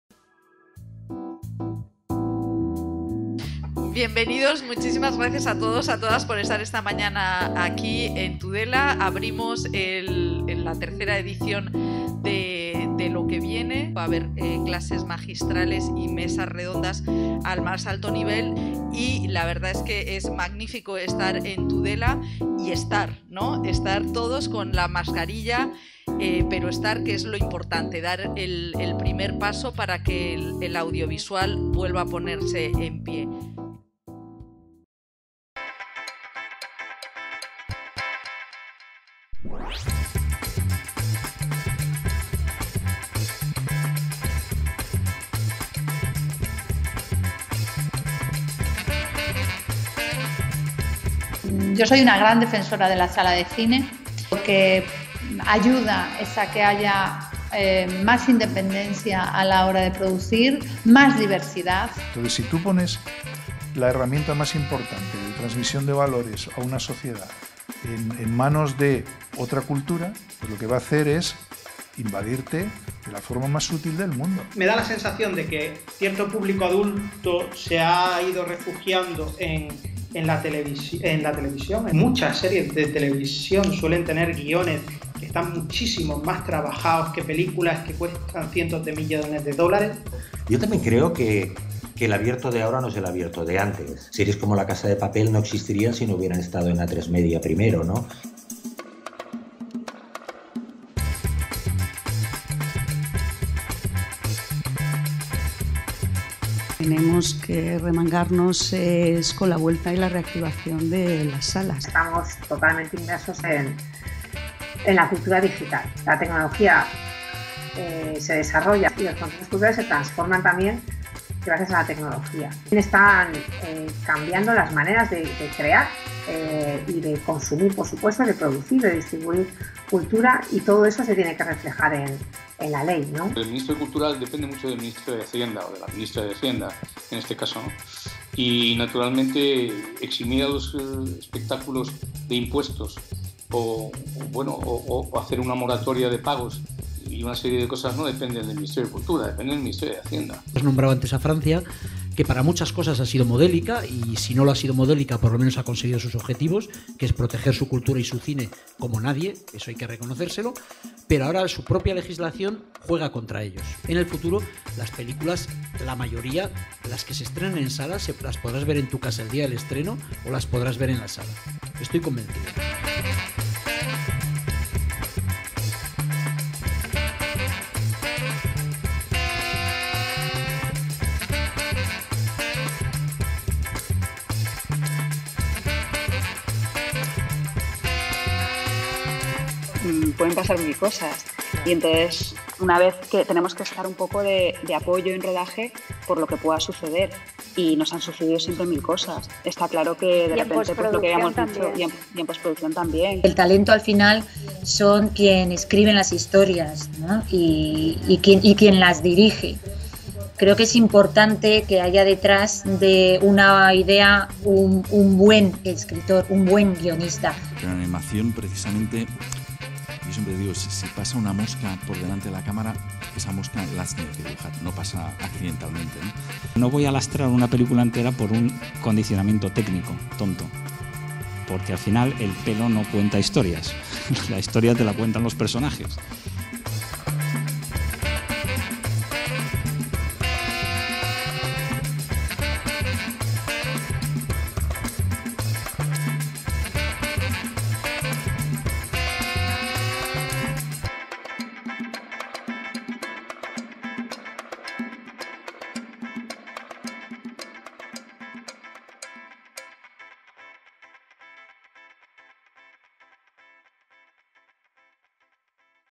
Resumen de LO QUE VIENE 2020
Vídeo con los mejores momentos del certamen de cine y series organizado por la Asociación de Informadores Cinematográficos de España (AICE) en la Ribera de Navarra del 15 al 17 de julio de 2020